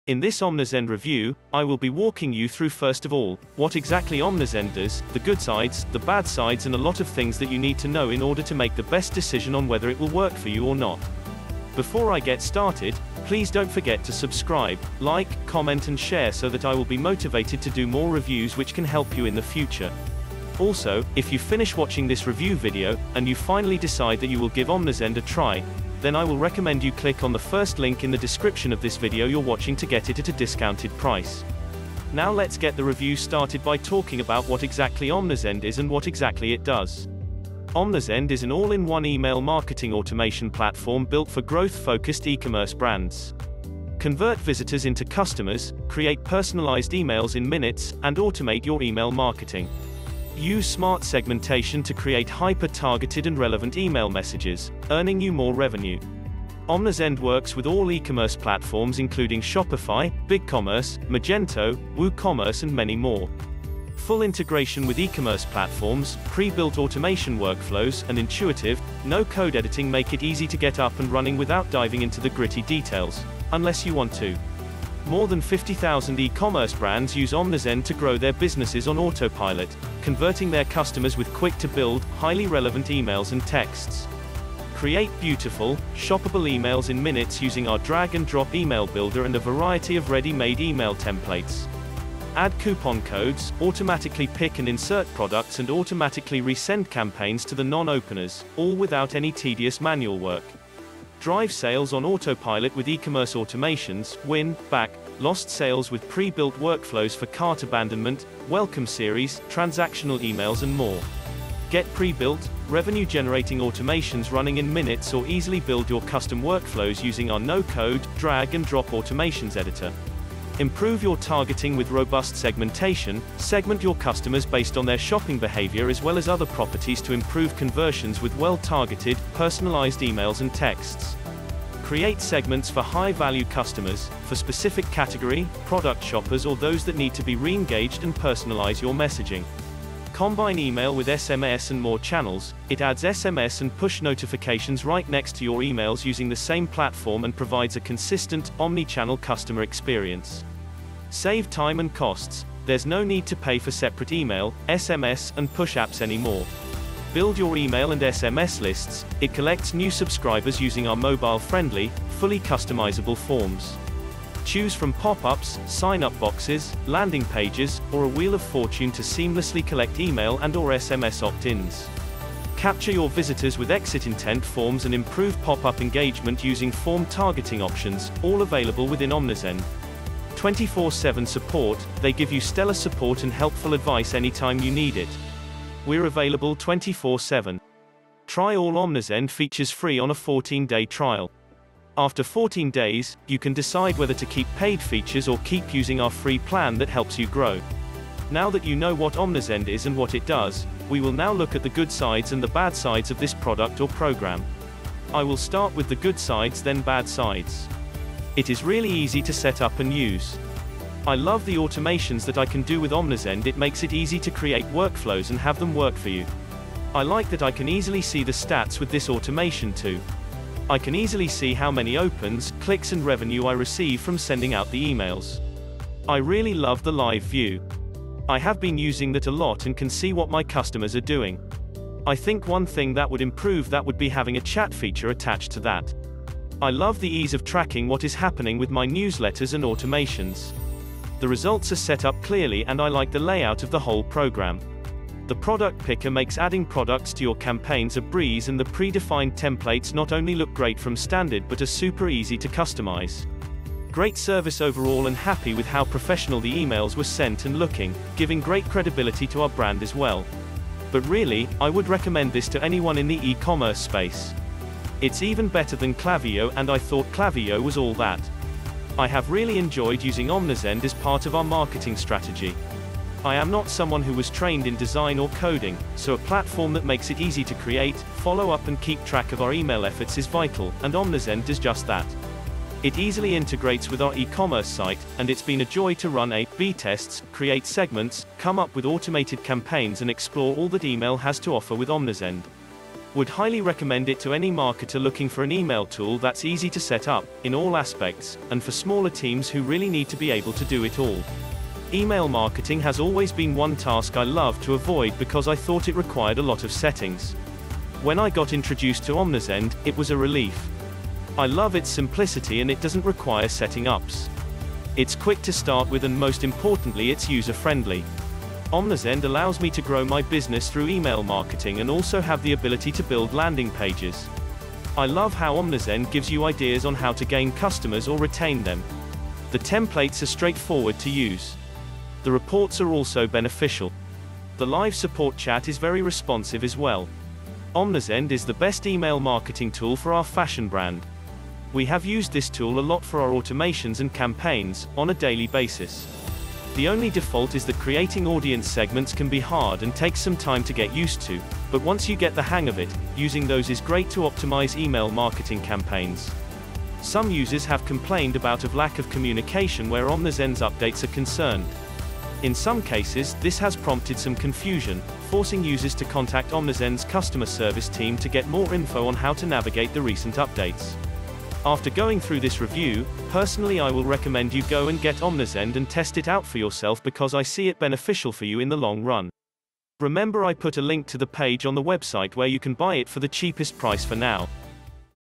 Omnisend Honest Review ❇️ eCommerce Email Marketing & Automation Platform 🔥
If you are looking for an easy eCommerce email marketing and automation platform to help increase your sales, then you need to watch this Omnisend review. Omnisend is a powerful omnichannel marketing platform to take your eCommerce business to the next level.

With a few clicks of a button inside of Omnisend, you can create email marketing automation for things like abandoned carts, re-engaging sequences, deals, welcoming messages, and other more automation workflows. You can also use SMS messaging, Push notifications, and pixel triggering for more in-depth and powerful marketing campaigns.

It's safe to say, eCommerce + Omnisend is a fast and easy way to step up your eCommerce marketing game.